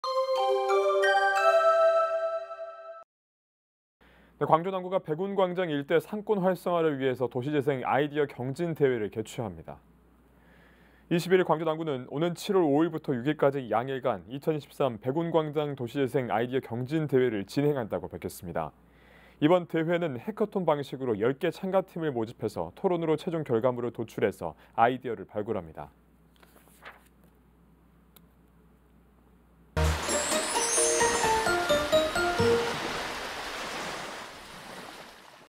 [광주뉴스] 광주 남구 백운광장 '아이디어 경진대회' 진행...참가팀 모집
■CMB뉴스

광주 남구가 백운광장 일대
상권 활성화를 위한 도시재생
아이디어 경진대회를 개최합니다.

21일 광주 남구는 오는 7월 5일부터 6일까지 양일간
'2023 백운광장 도시재생 아이디어 경진대회'를
진행한다고 밝혔습니다.

이번 대회는 해커톤 방식으로 10개 참가팀을 모집해
토론으로 최종 결과물을 도출해서
아이디어를 발굴합니다.